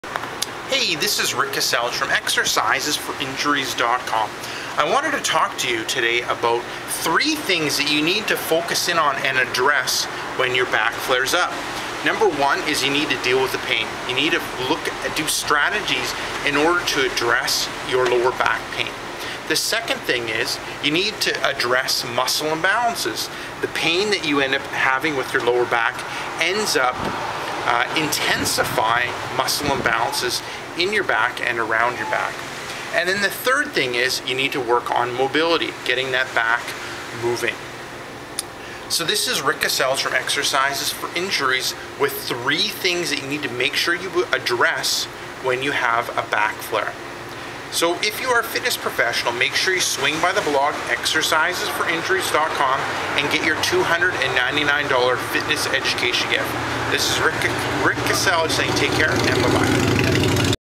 3 Things You Need to Do When You Have a Lower Back Pain Flare Up
to get your Shoulder Injury Guide

I few weeks back I had a bad lower back pain flare up.